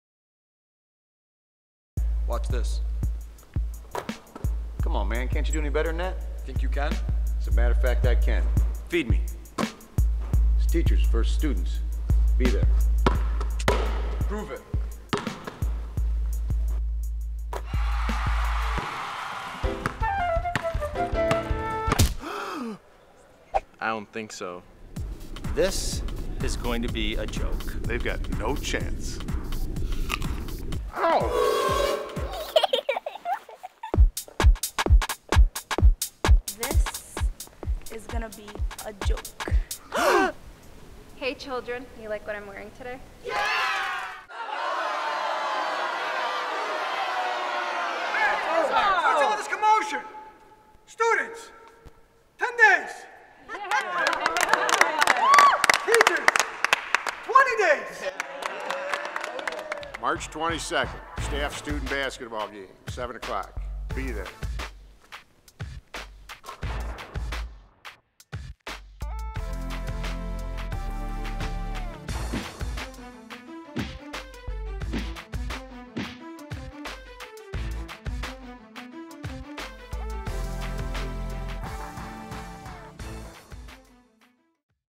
DHS Staff vs. Students Basketball Game
The 1st Annual Dearborn High School Staff vs. Students Basketball Game will be held on Thursday, March 22, at 7 p.m. at Dearborn High School (19501 Outer Drive, Dearborn, MI, 48124).  Come watch the Varsity basketball players from both the Men's and Women's teams battle it out with Staff of Dearborn High! This is sure to be an extremely entertaining and enjoyable event for all. Don't miss it!

Admission is $5. No athletic passes accepted.